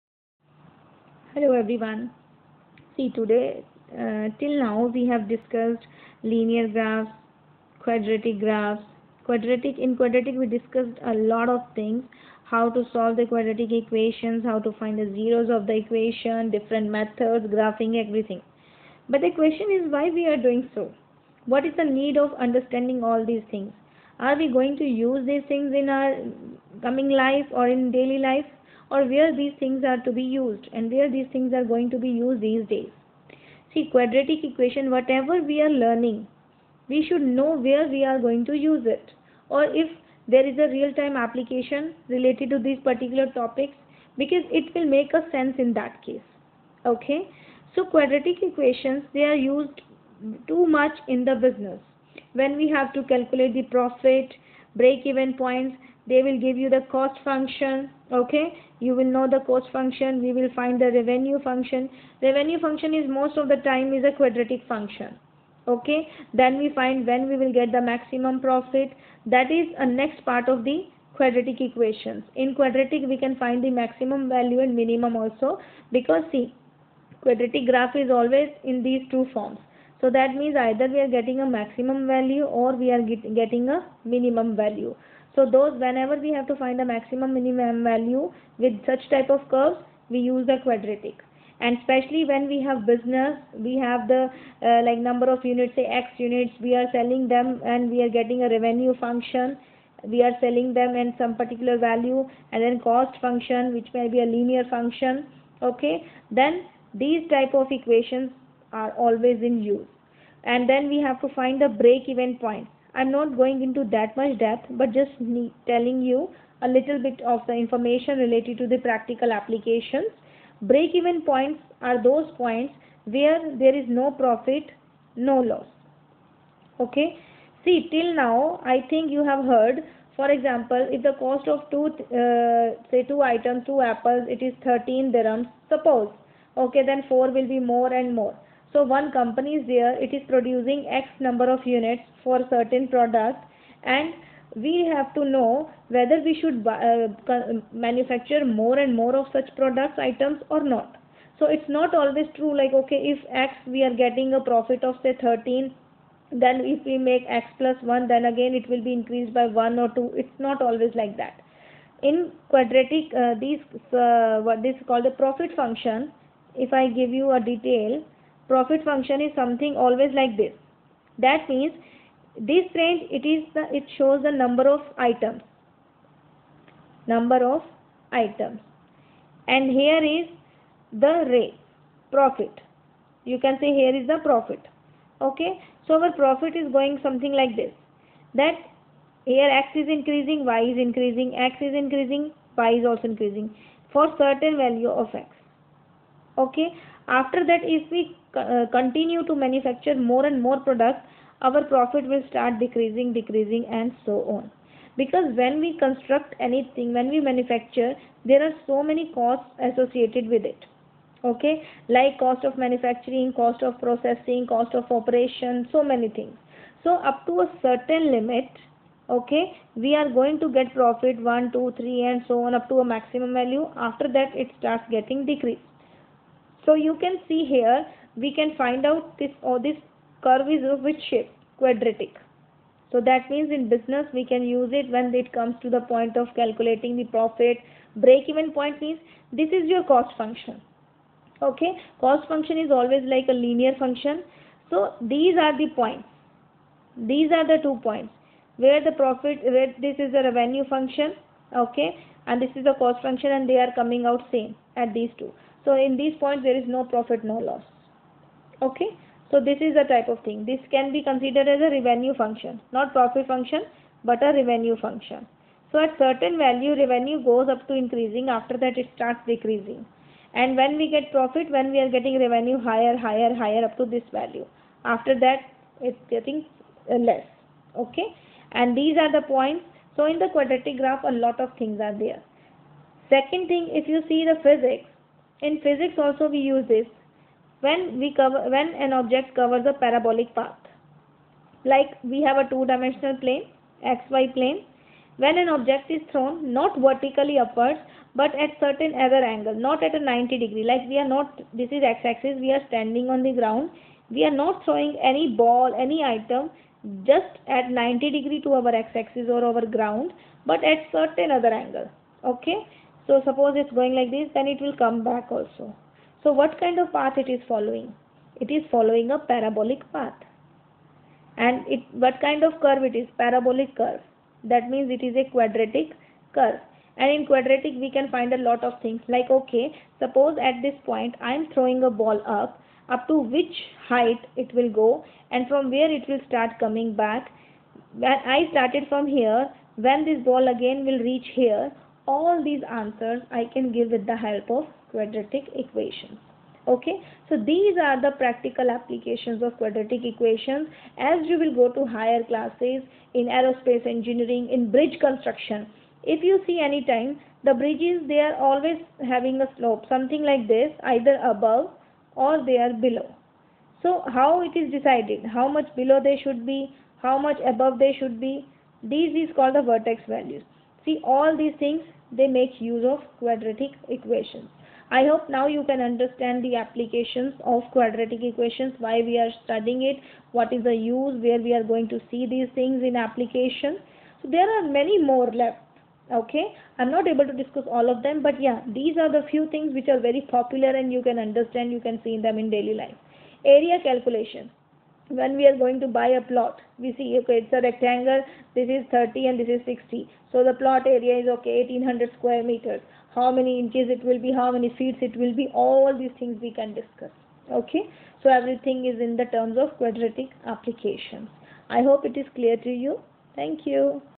Real application of quadratic equations
In business , parabolic path, economy, supply n demand area